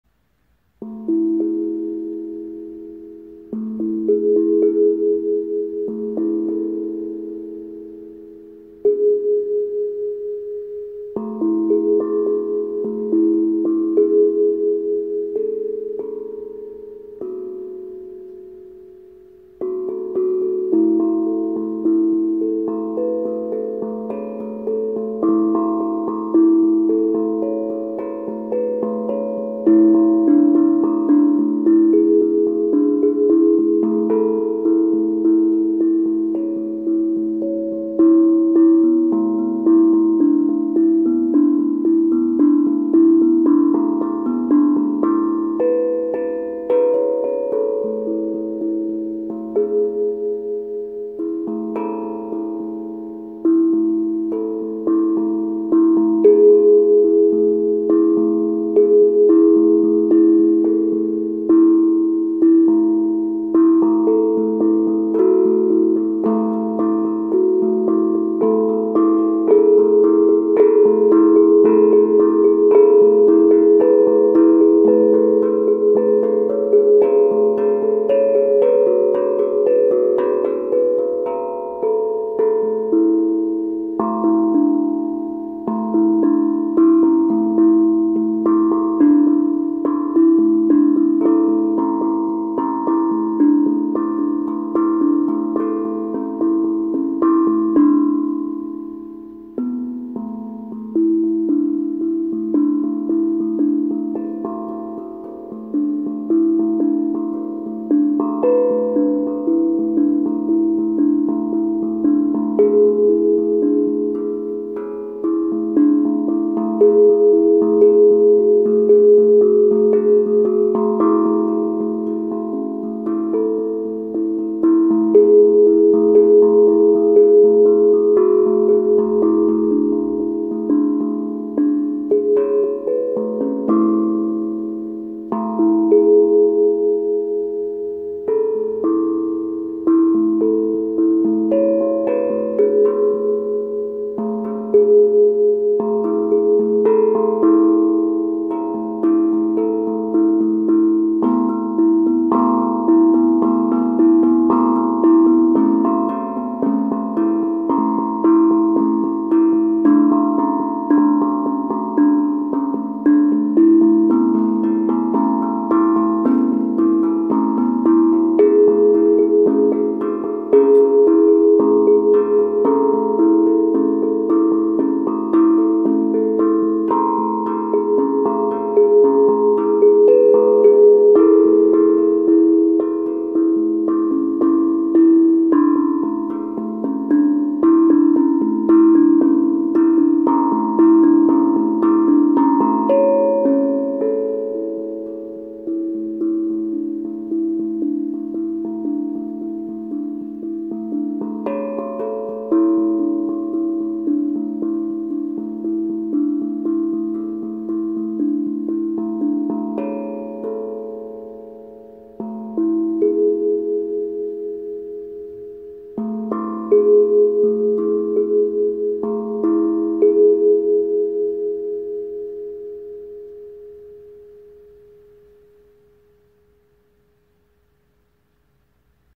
Improv 1,206: Spacey (Artisan Instruments tongue drum)
This one came out kinda outer-spacey. The fact that I expect you all to know what I mean by that is interesting. How have we developed a dedicated "outer-spacey" musical sound?